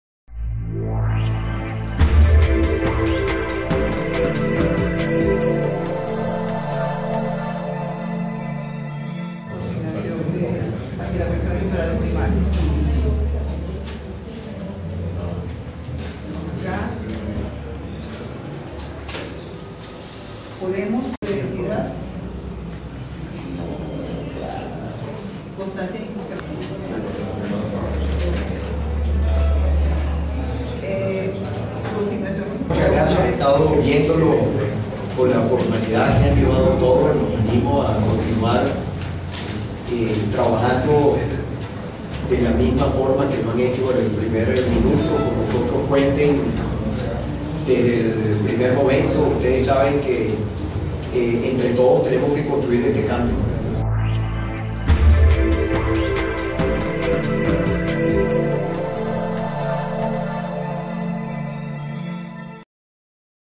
EXCLUSIVA Henrique Capriles entrega documentos y se dirige a la CEP
Los partidos postulantes entregan los requisitos a la Comisión Electoral de la Unidad para la inscripción a las primarias de Henrique Capriles Radonski a las primarias. El Gobernador de Miranda se dirigió a la CEP para ratificar su participación en la Unidad ya que "entre todos debemos construir este cambio"